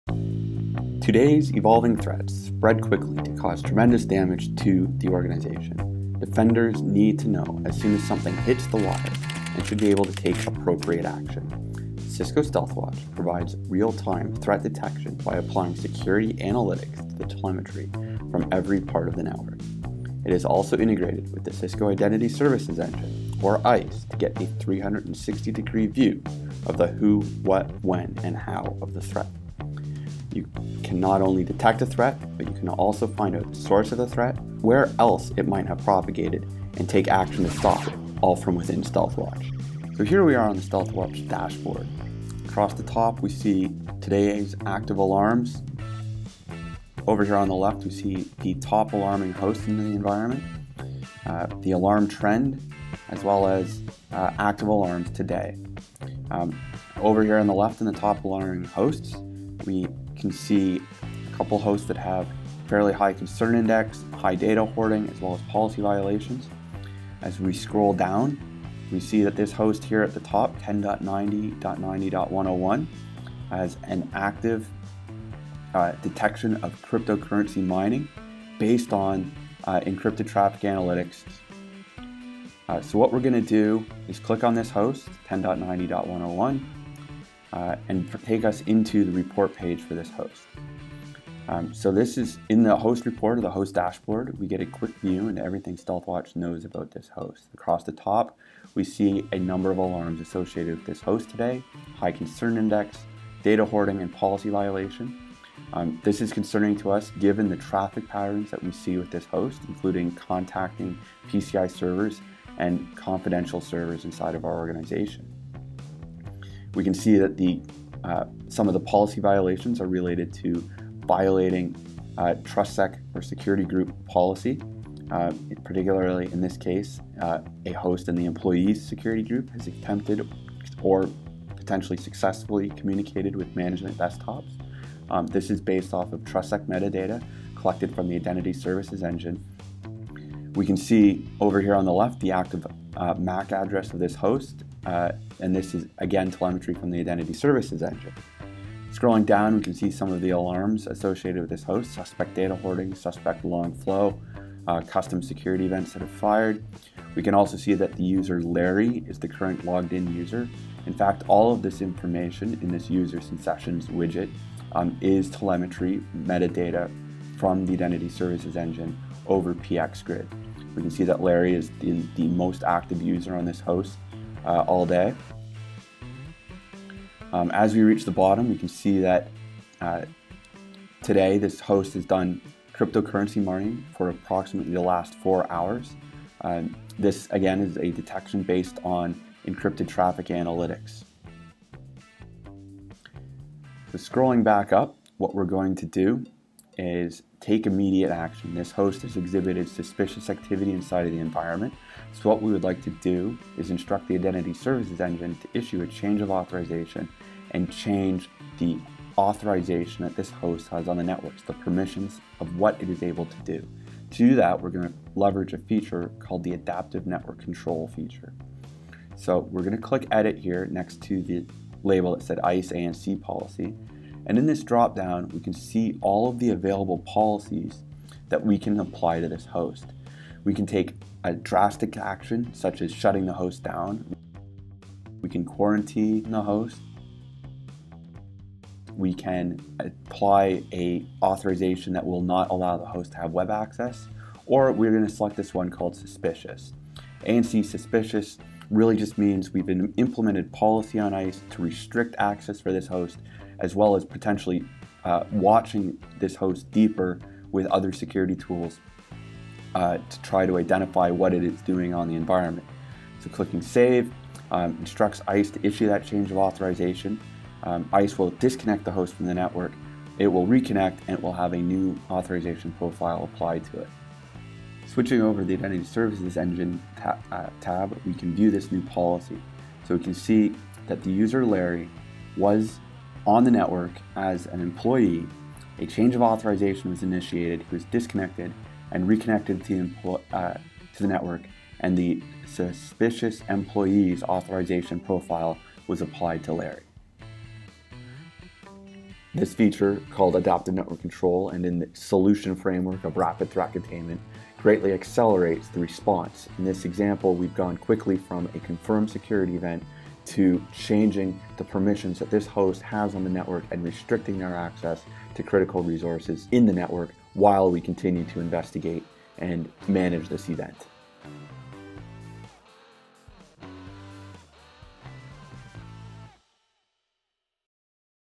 Context-aware threat mitigation with Cisco Stealthwatch and Identity Services Engine (ISE)
This demo covers the latest enhancements to the Stealthwatch integration with ISE. ISE provides additonal user and identity context to a threat detection within Stealthwatch. And now, leveraging the ISE Adaptive Network Control (ANC) policies, users can choose from multiple mitigation options and take action on the infected host based on the severity of the threat.